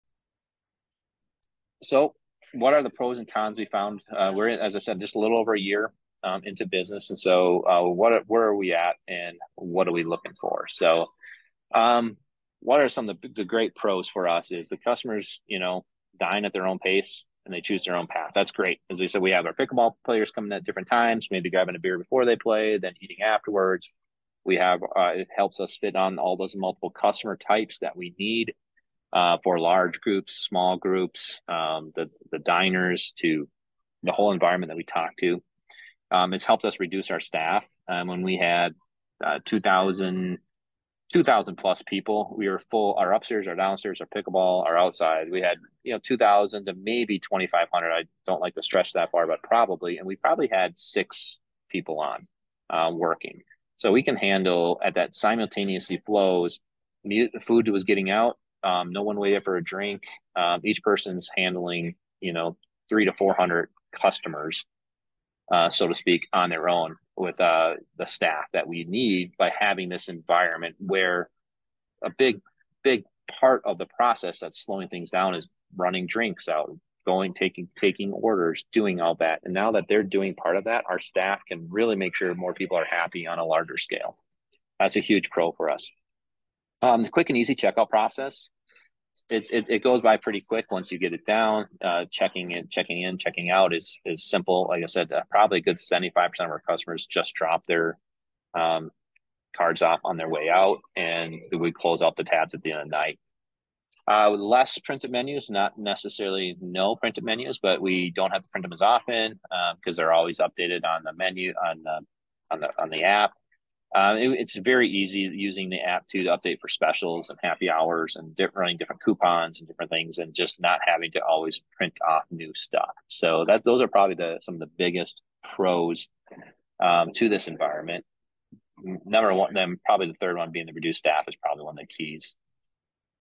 Pros and Cons of Hybrid Service Pros
Lean about the pros and cons of hybrid service models in eatertainment/hospitality venues.